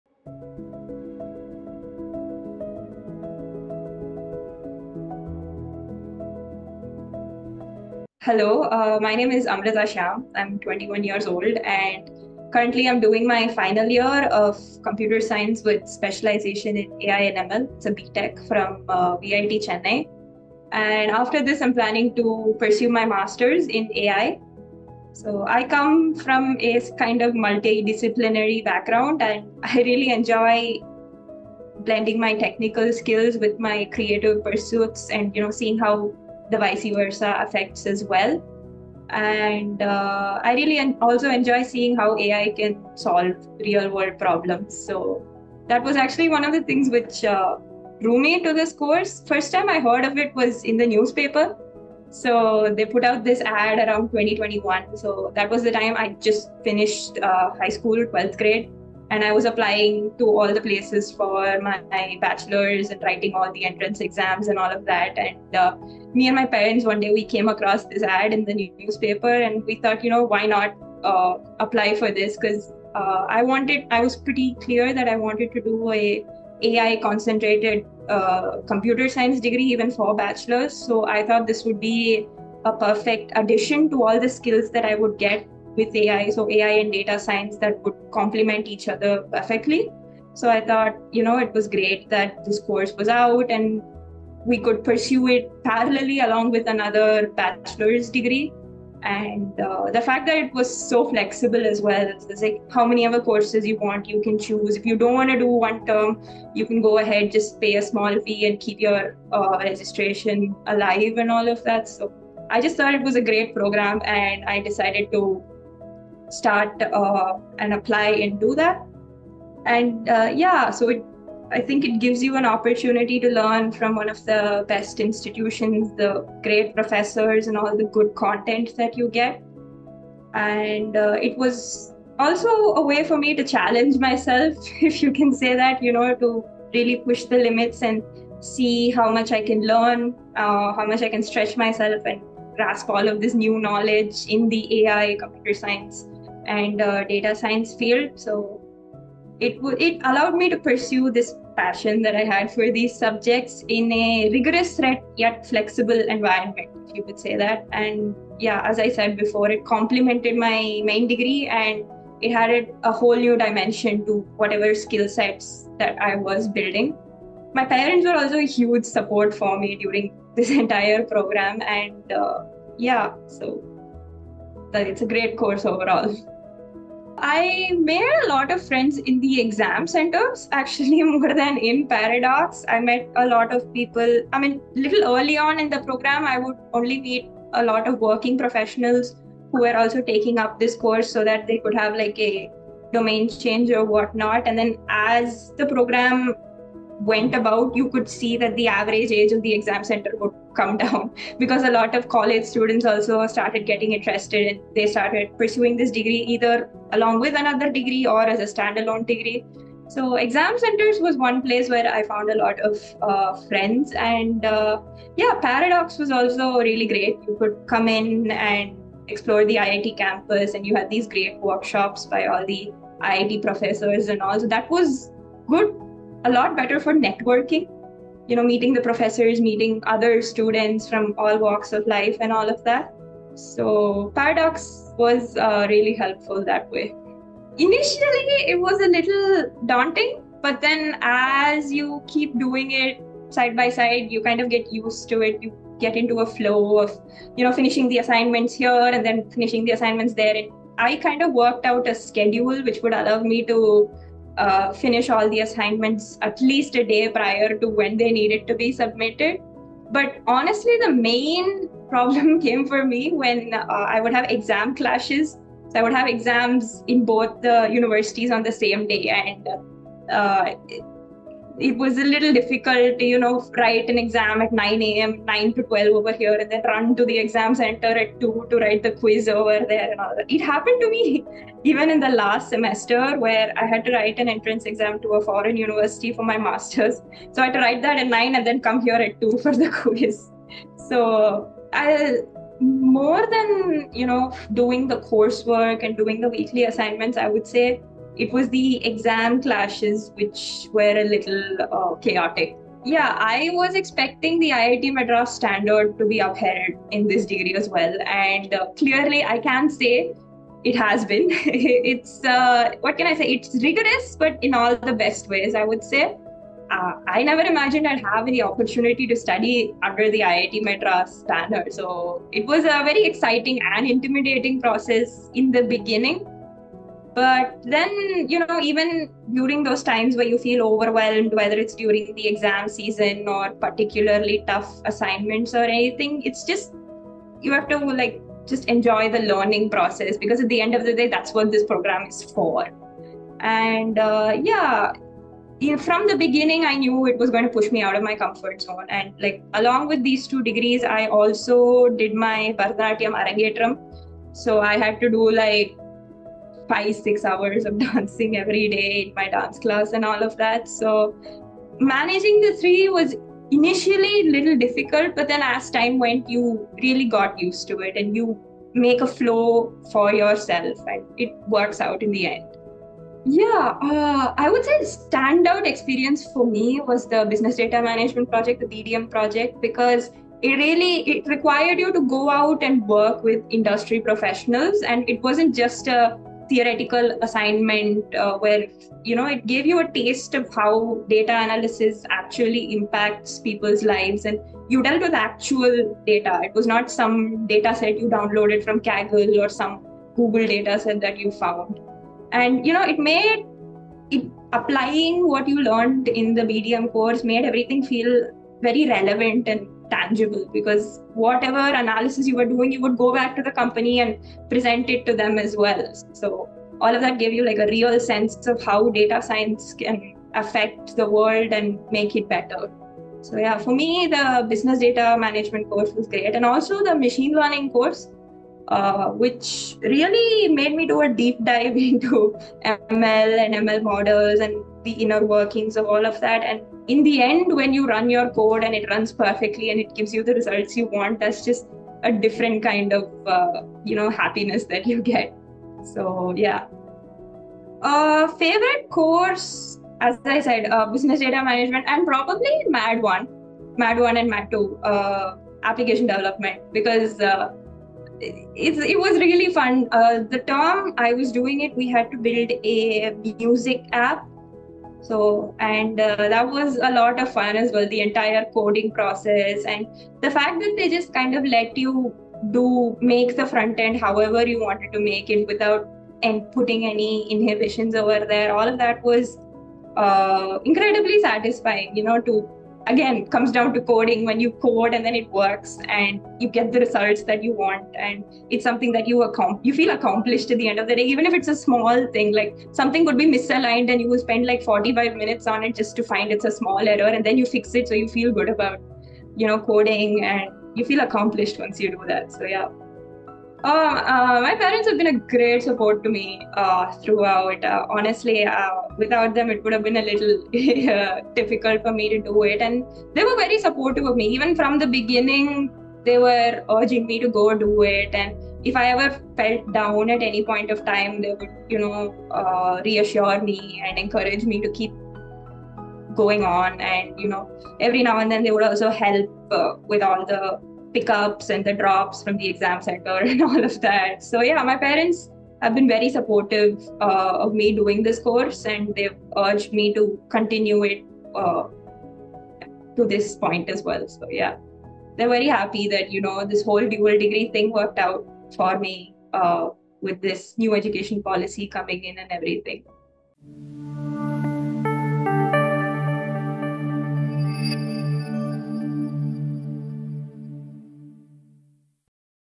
Dancer Amrutha Shyam found her AI spark in IIT’s BS program—fueling both passion and creativity!
Dancer Amrutha Shyam now thrives in AI, thanks to IIT's BS program. She champions its multidisciplinary flexibility, which, though initially daunting, let her flourish. This environment perfectly channeled her creative drive into the future of AI."